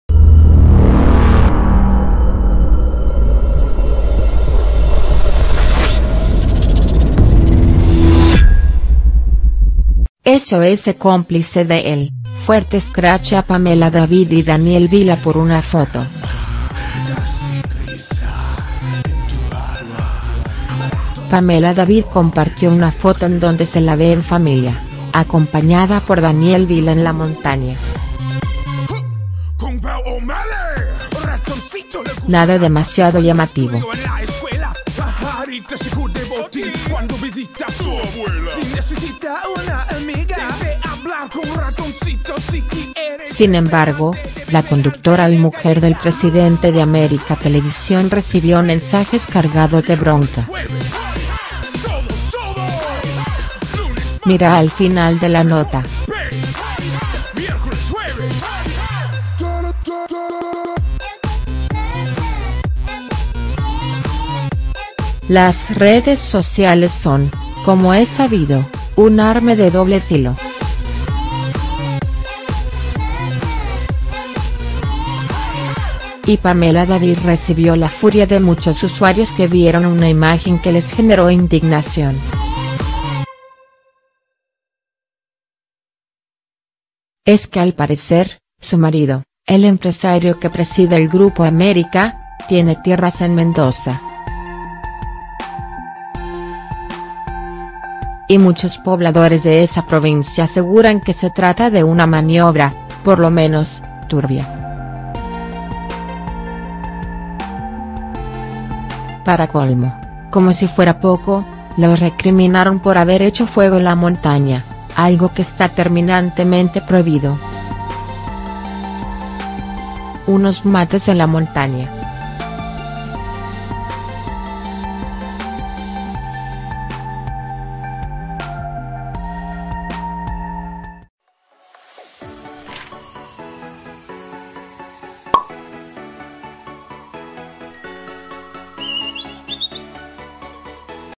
"Sos cómplice de él": fuerte escrache a Pamela David y Daniel Vila por una foto
La conductora de América y su marido, dueño del canal y de otros medios del Grupo América, posaron para una postal que parecía de lo más inocente. Sin embargo, hizo explotar a algunos